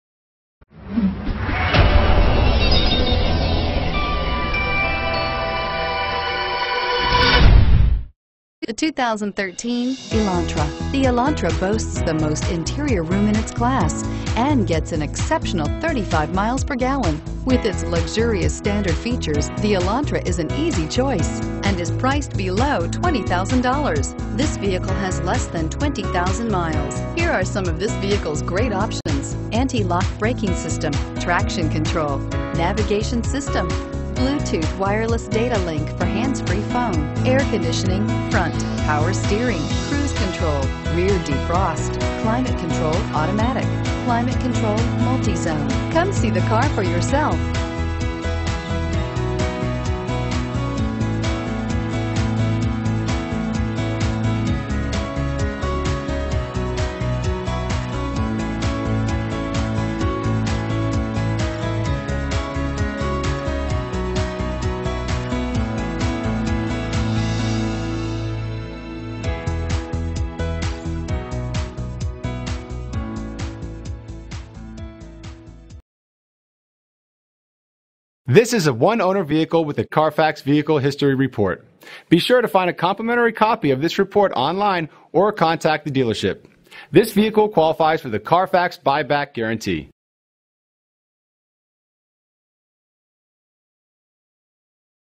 2013 Hyundai Elantra Melbourne FL H39459A
2013 Hyundai Elantra Limited

Coastal Hyundai
915 New Haven Avenue W

Silver Bullet! Move quickly! Remainder of the 5 year 60,000 mile Bumper to Bumper warranty and Roadside assistance. Want to save some money? Get the NEW look for the used price on this one owner vehicle. Previous owner purchased it brand new! Suspension system provides a quiet ride. The Elantra is named as AUTOSPIES.com Car of the Year. We only keep the CLEAN CARFAX cars, we sell the others to our competition.